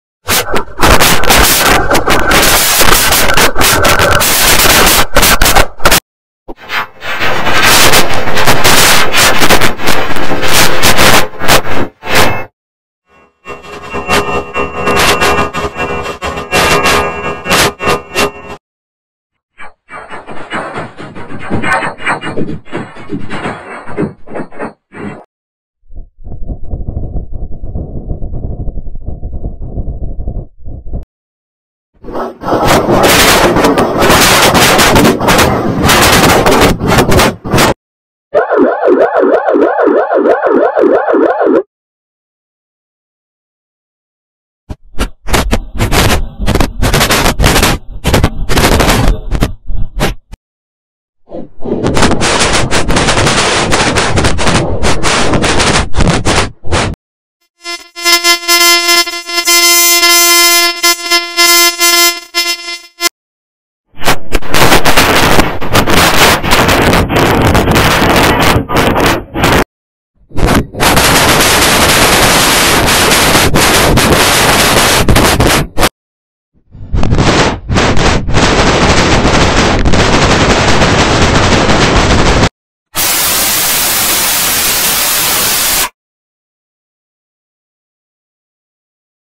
Klasky Csupo Robot Logo Effects Round 28 Vs MediaStanRay/MSR (28/100)
Round 28 Effects List:
Luigi Group
Scary X
I Killed X
Does Respond
Bug Major
Happy
X Is Going Into A Portal
Pinch Wacky
763ormulator V74
Performance Major
Random Effect
X Is Having Some Problems